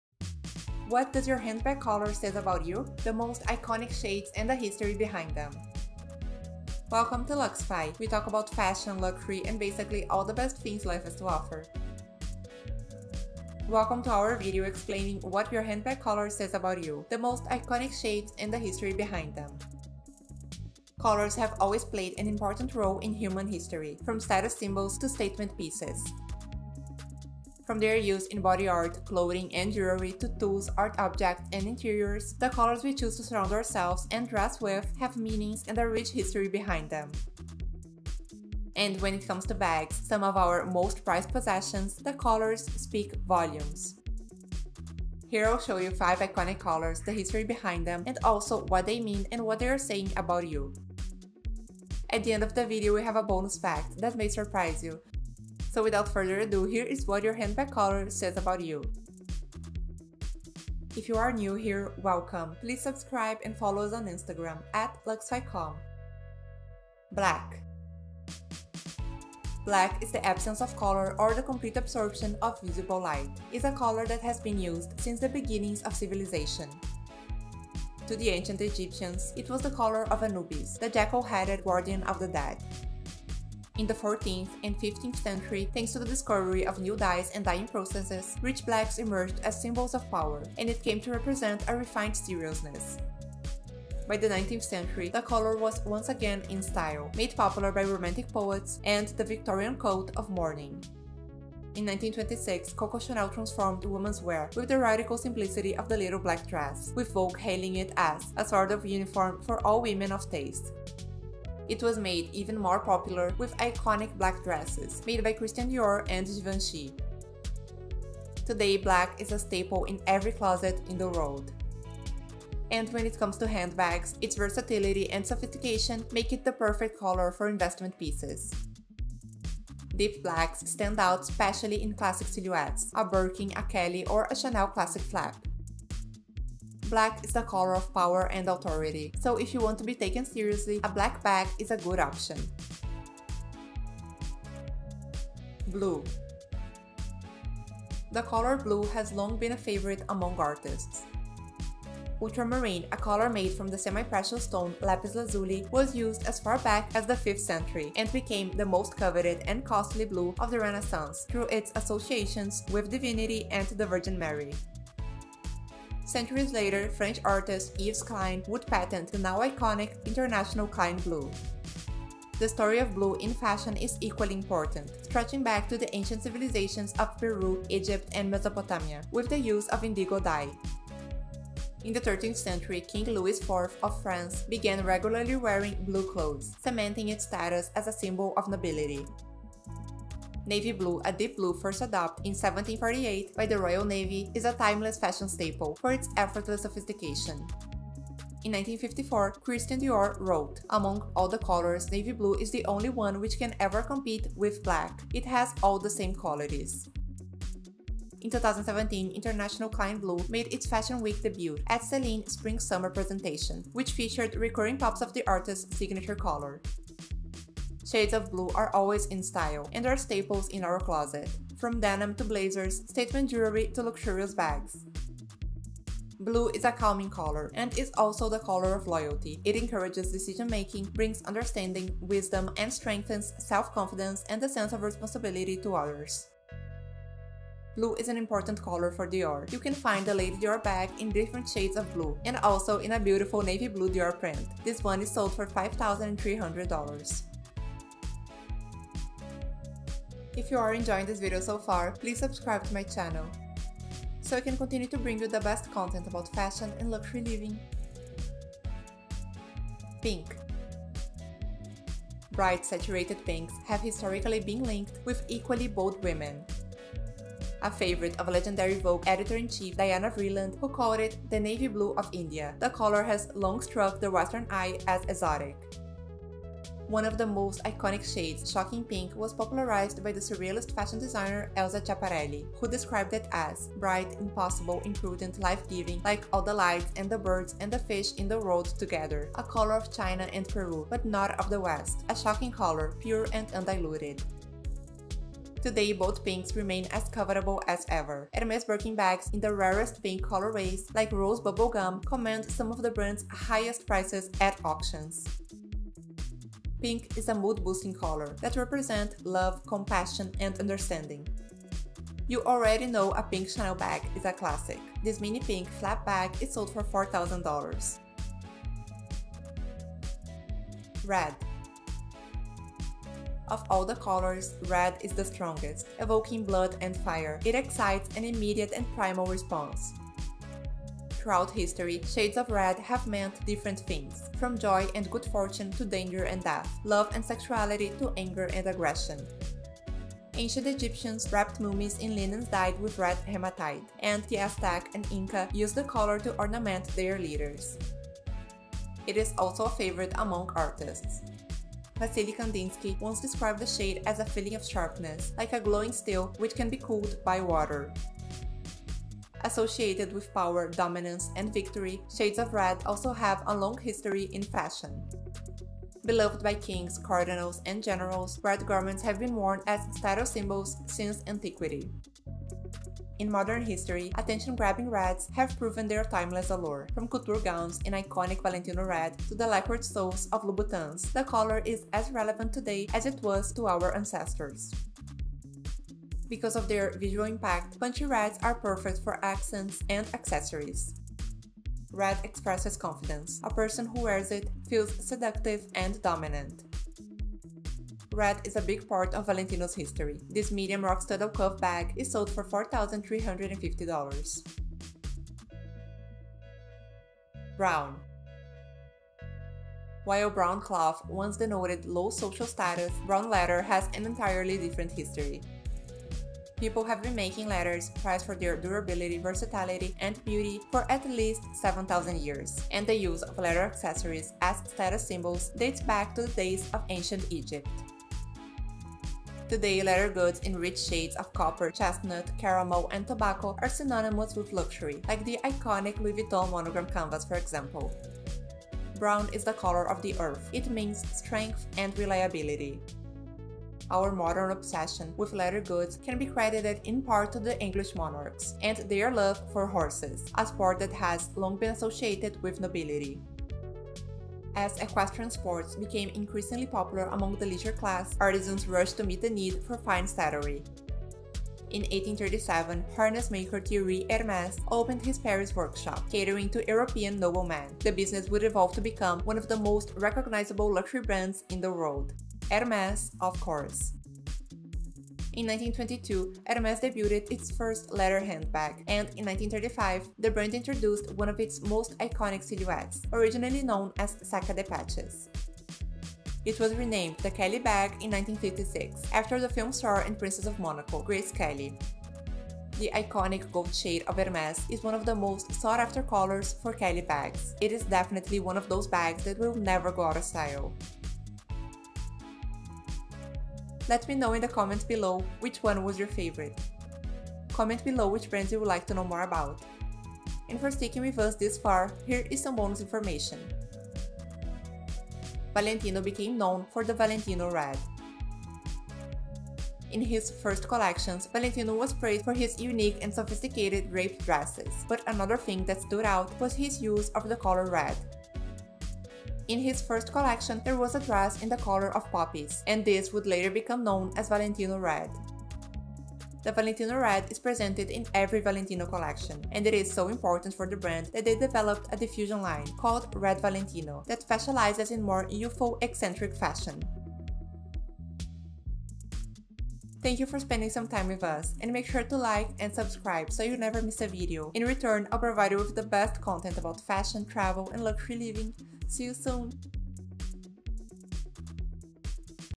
What Does Your Handbag Color Say About You? The Most Iconic Shades and the History Behind Them
Welcome to our video explaining What Does Your Handbag Color Says About You? The Most Iconic Shades and the History Behind Them!

Colors have always played an important role in human history: from status symbols to statement pieces. From their use in body art, clothing and jewelry to tools, art objects and interiors, the colors we choose to surround ourselves and dress with have meanings and a rich history behind them.

And when it comes to bags, some of our most prized possessions, the colors speak volumes! Here I’ll show you 5 iconic colors, the history behind them and also what they mean and what they are saying about you!

At the end of the video we have a bonus fact that may surprise you!

00:00 - Intro
01:06 - Black
02:25 - Blue
04:19 - Pink
05:20 - Red
06:47 - Brown
08:35 - Bonus Fact